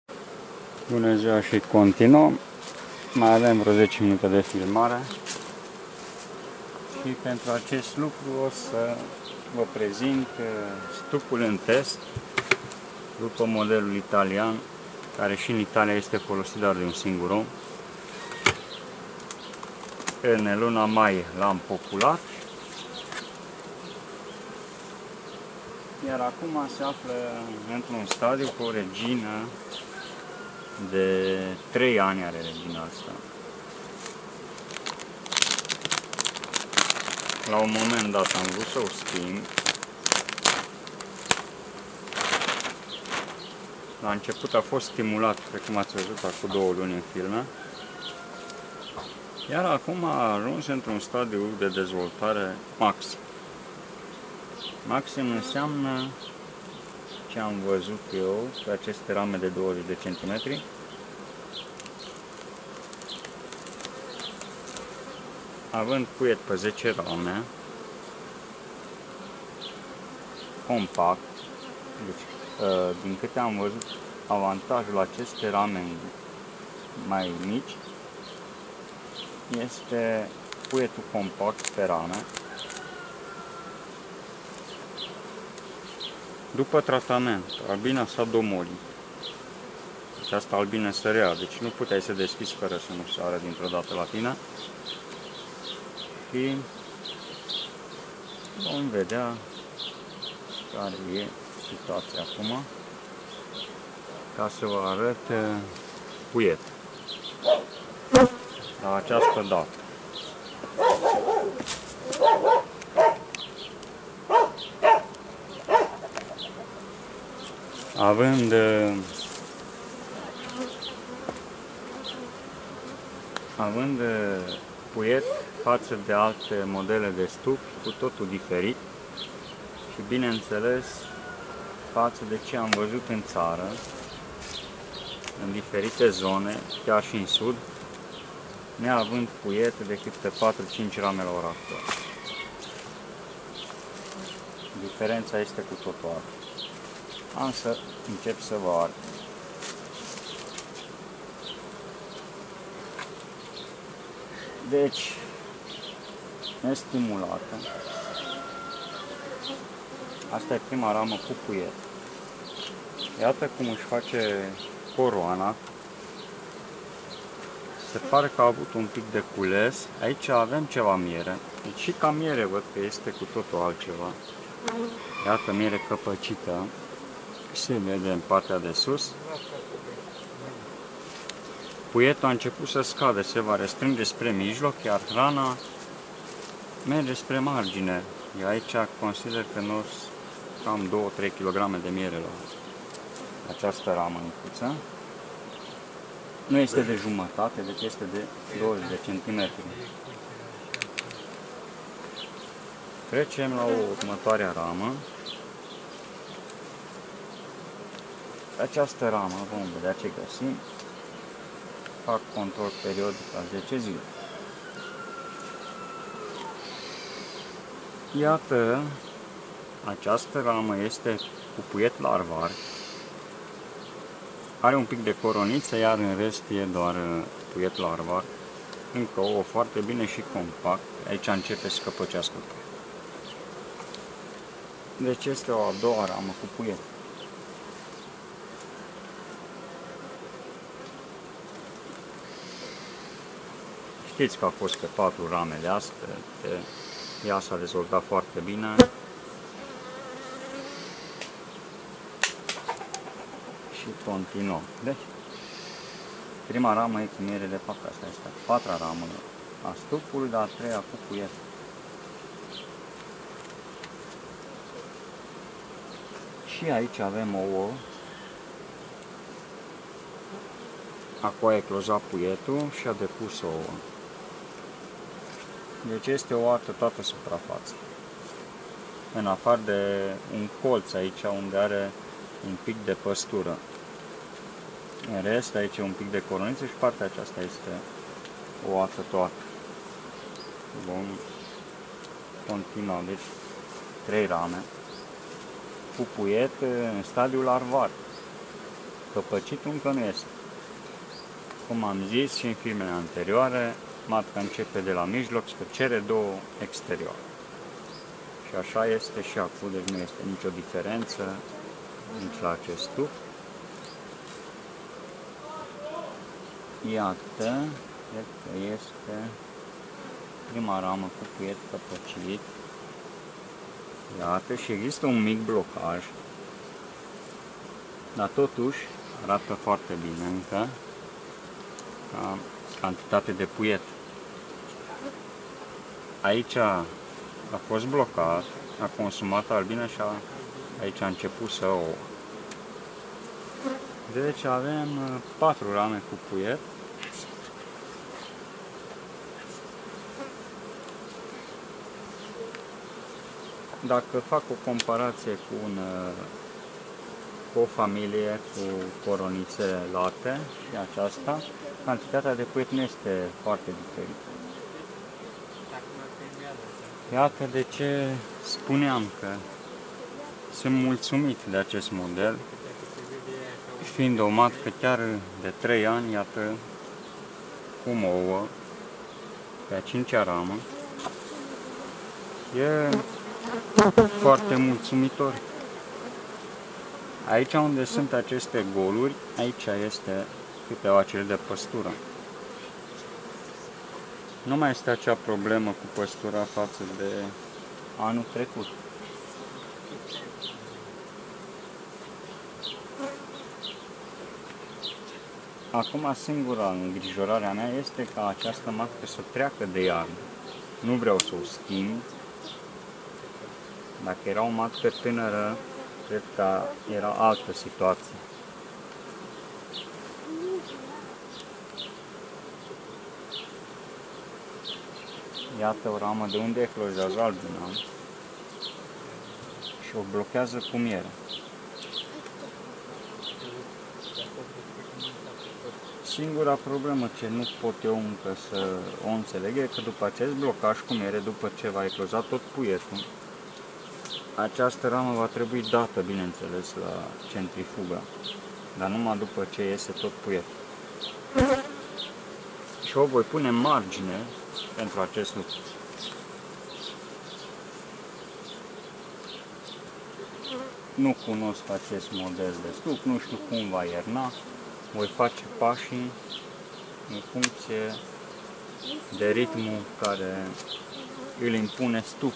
Lucrari in stupina , stupul in teste arata bine , are puiet suficient pentru aceasta data
Iata acest stup arata foarte bine , au trecut cam 3 luni de la popularea lui , puietul este ok la aceasta data .